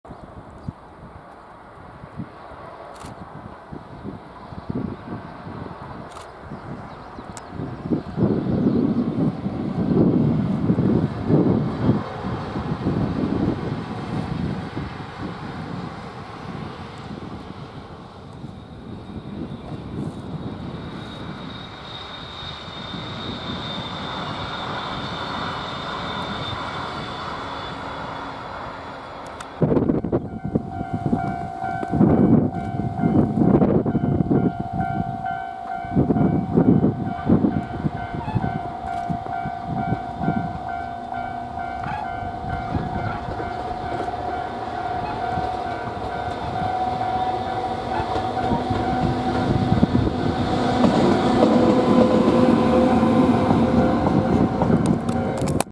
【GW明け、予讃線を一両で走るEF210】
2022年５月９日、ゴールデンウイークが終わり、所定の貨物列車のダイヤでEF210の単機回送が運行され、予讃線を下りました。撮影は3071レです。JR四国位置情報アプリでも、貨物列車ではなく回送列車と表示されます。